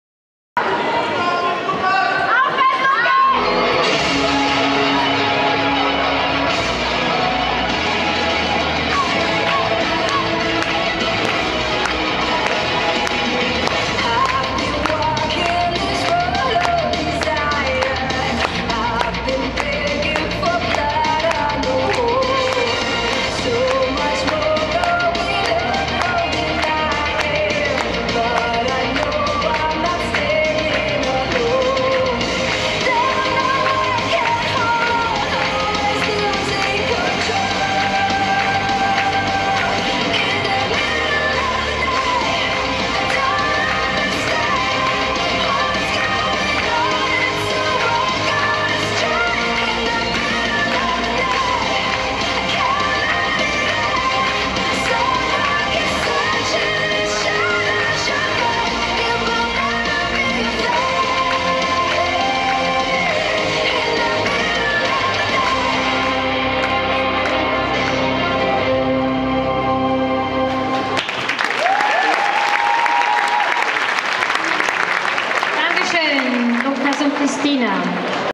Christina BISCHOFF-MOOS und Lukas MOOS Rock´n´Roll WM Winterthur 9.11.2013 Fußtechnik VR
Rock´n´Roll A-Klasse 18. Platz